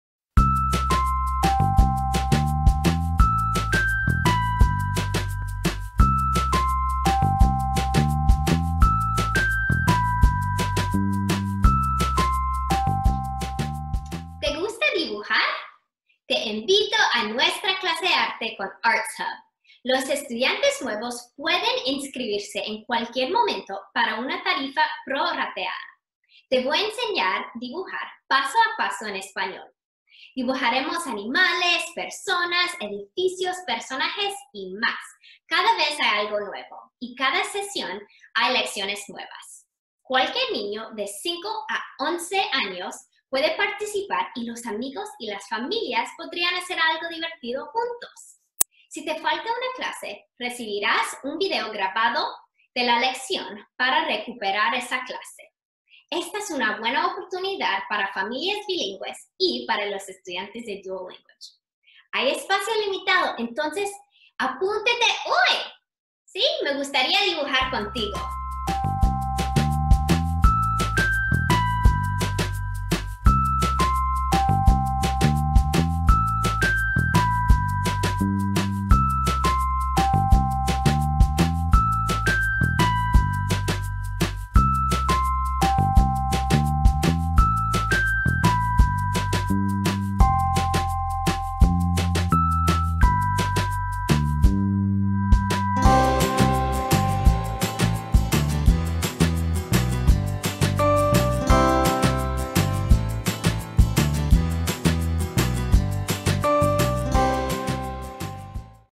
Weekly Virtual Art Classes In Spanish For Ages 5-11
Any child ages 5-11 from anywhere may participate in ArtzHub weekly virtual art classes.

Wednesdays, 3:30-4:30 EST is taught in Spanish by Amanda Brady on Zoom.

Session 3: 1/20/21-3/10/21
Session 4: 3/17/21 - 5/12/21

New students may join at any time for a pro-rated fee.